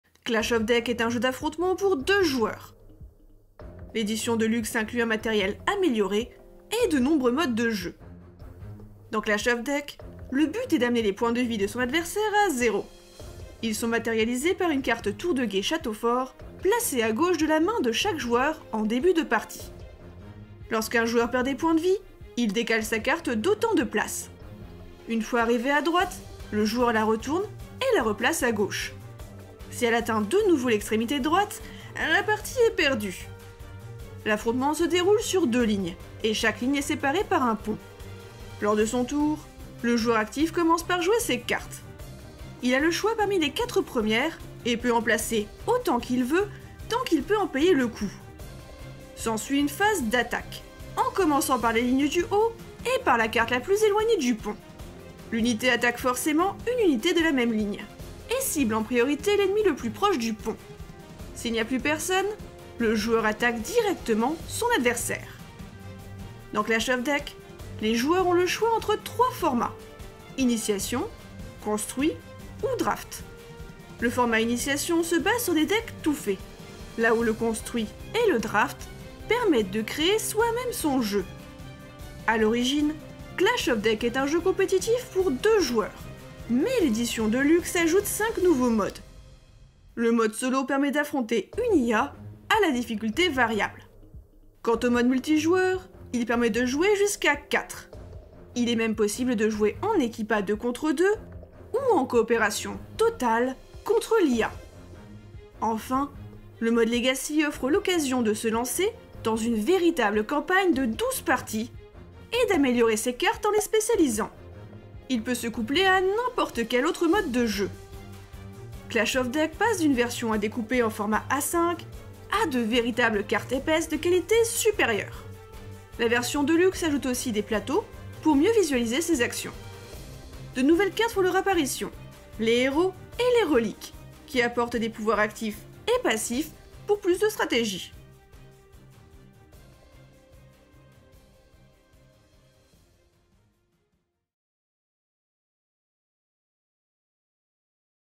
Clash of Deck : Deluxe Edition - Présentation du jeu
Clash of Deck est un jeu d’affrontement pour 2 joueurs. L’édition Deluxe inclut un matériel amélioré et de nombreux modes de jeu.

Informations sur le jeu :
●  30 minutes
● Auteur : Léandre Proust
● Illustrateur : Clément Proust
● Editeur : Grammes Edition